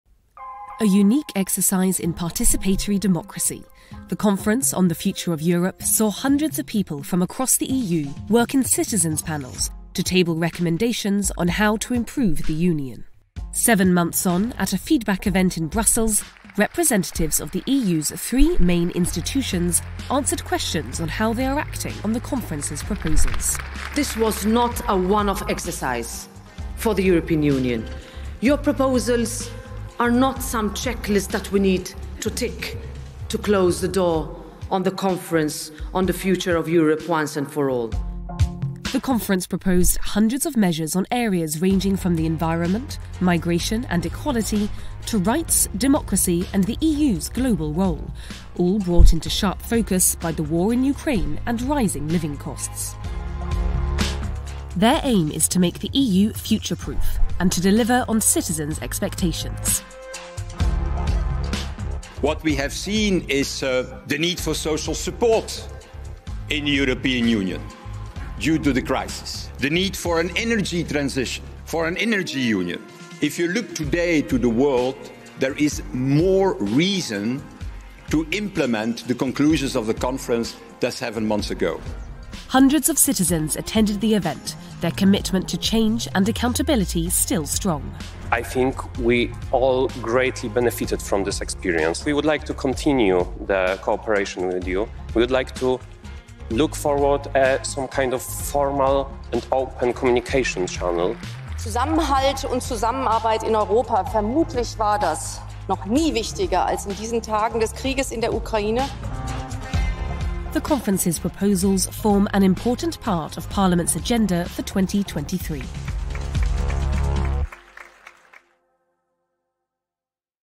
Viitorul UE: cetăţenii cer mai mult de la UE
Cetățenii europeni care au fost în centrul activității Conferinței privind Viitorul Europei au venit la Bruxelles, la începutul lunii decembrie, pentru a analiza modul în care instituțiile UE urmăresc recomandările lor de schimbare. Un exercițiu unic de democrație participativă, Conferința s-a încheiat în luna mai 2022, adunând zeci de propuneri ale cetățenilor care să asigure că Europa poate face față provocărilor viitoare.

Parlamentul, Consiliul și Comisia au promis încă de la început să dea curs propunerilor finale, oricare ar fi acestea. Pe 2 decembrie, reprezentanții celor trei instituții au răspuns la întrebările cetățenilor la un eveniment de evaluare care a avut loc la Parlamentul European de la Bruxelles.